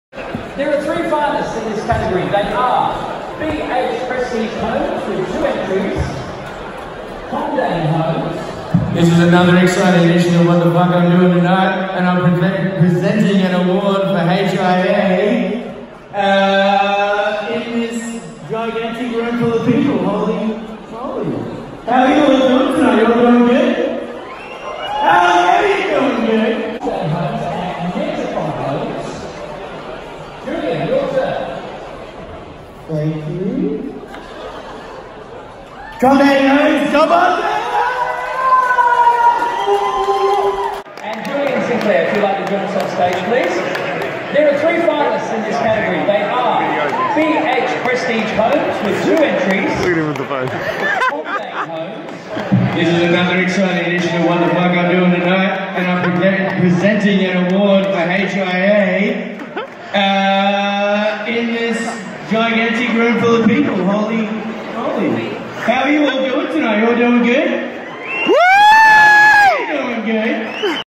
HIA AWARDS TIME
Just another exciting adventure of wtf I’m doing today and it’s HIA awards time and I have to do as speech